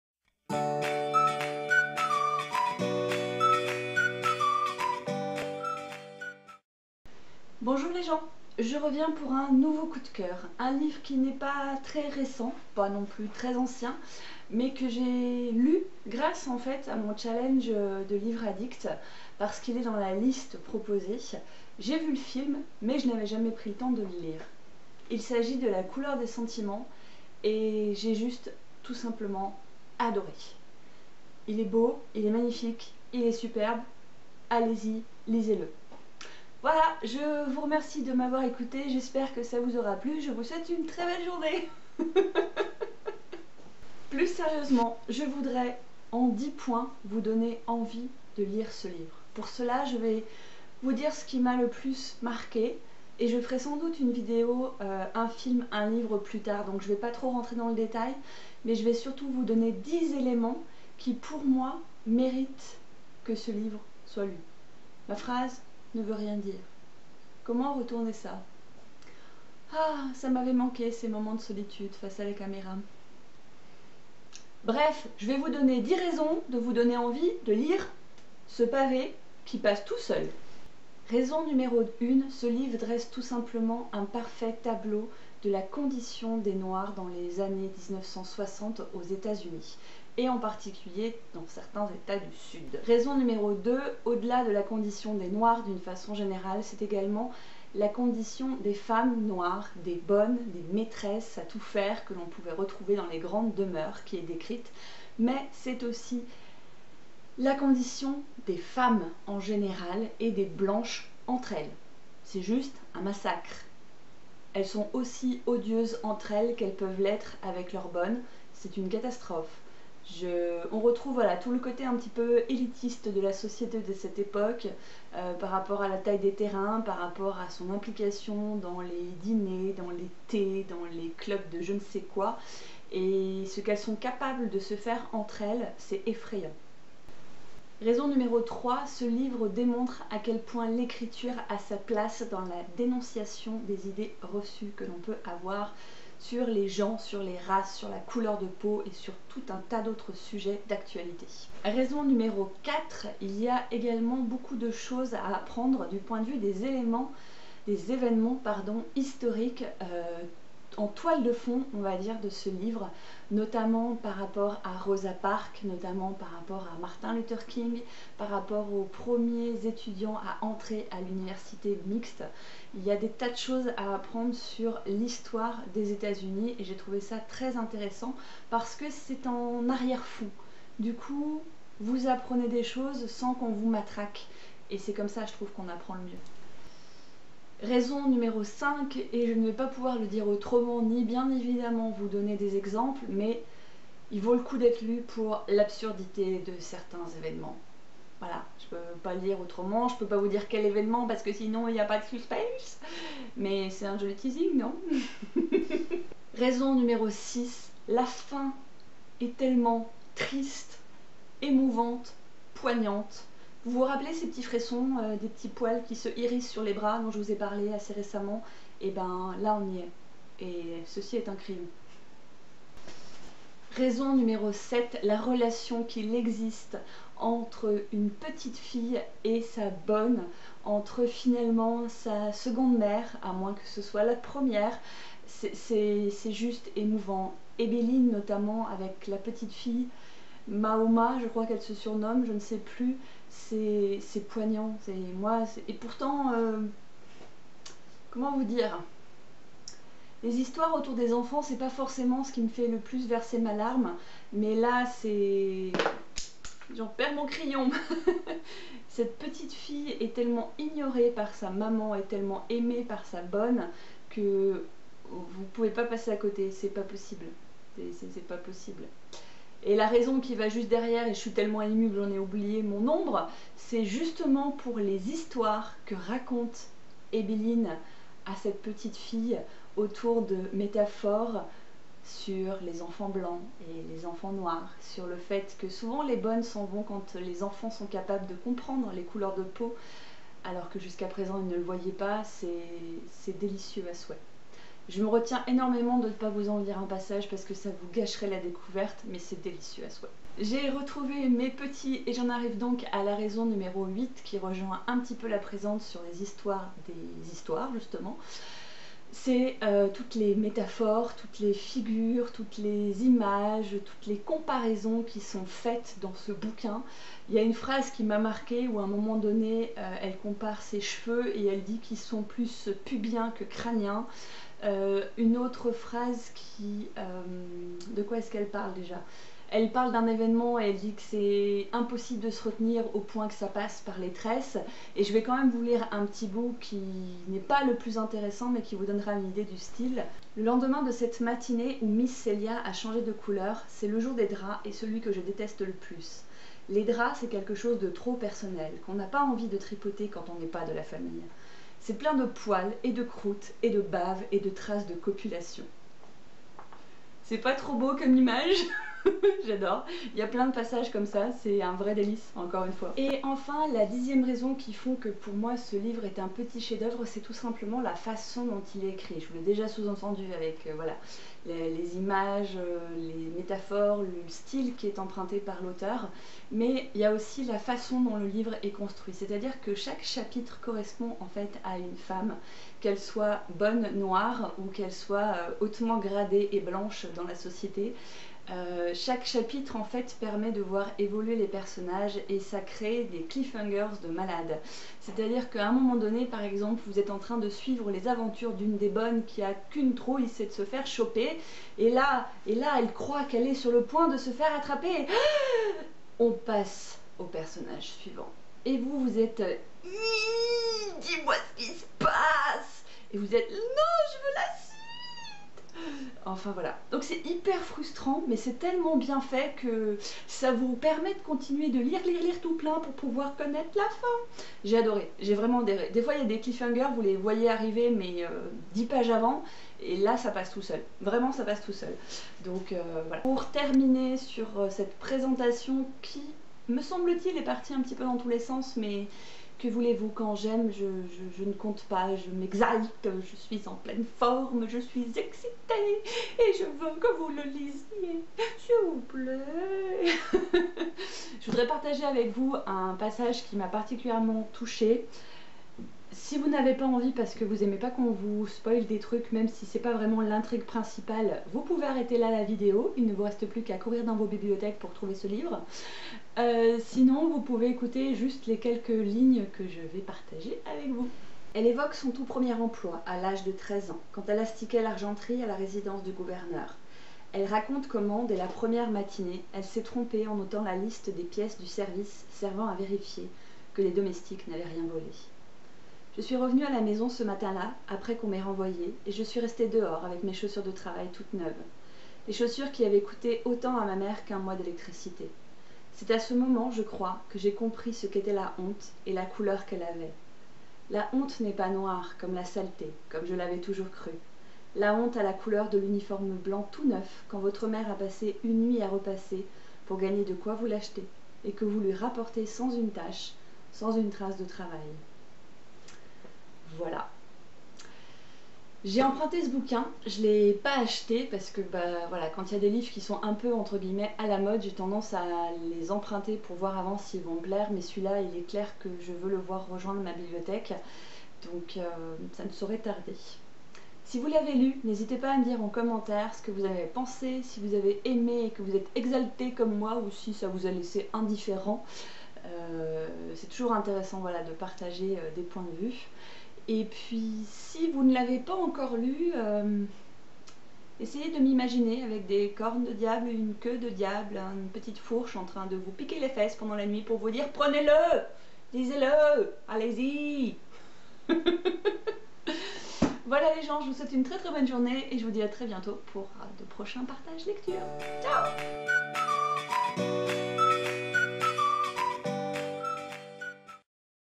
Livre Coup de Coeur | La couleur des sentiments (Kathryn Stockett)
Bonjour les gens !
10 mn pour vous partager 10 raisons de lire ce livre !

Je vous retrouve pour un nouveau coup de coeur.
La couleur des sentiments est un livre à mes yeux magnifique, plein de... plein de choses ! De l'amour, de l'amitié, de la tromperie, de l'Histoire, de la peine, de l'espoir... J'ai passé un merveilleux moment de lecture et ça me tenait aux tripes de le partager avec vous pour, peut-être, vous donner envie de le lire à votre tour !